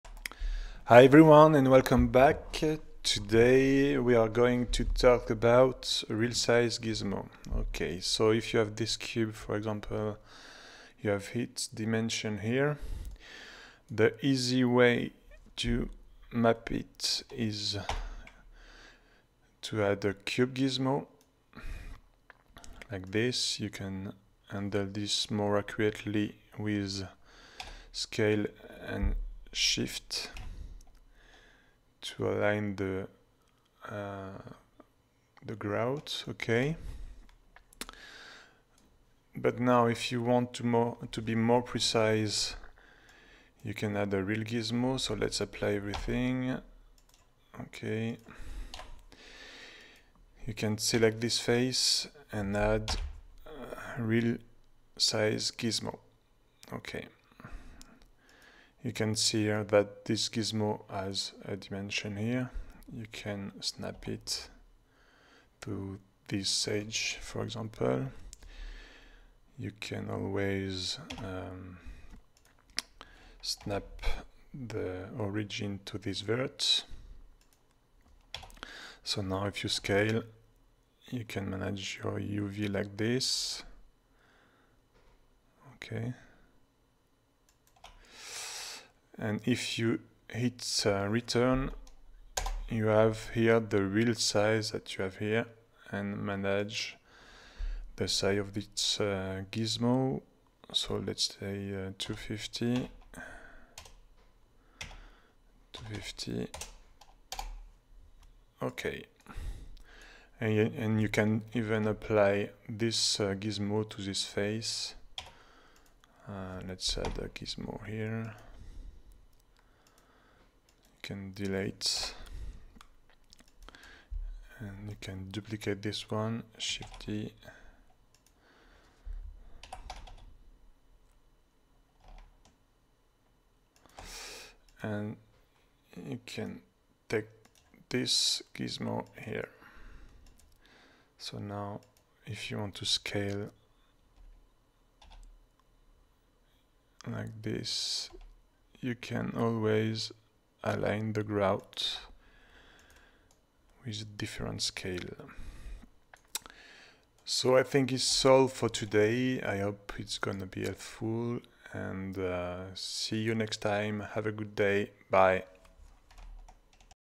Quick Uv Pro - Blender - Tiling with Real Scale Gizmo
Handle all your uvs with a Real Scale Gizmo allows you to be more accurate with tile layout. You can Snap it to your Geometry then, scale, move  and rotate.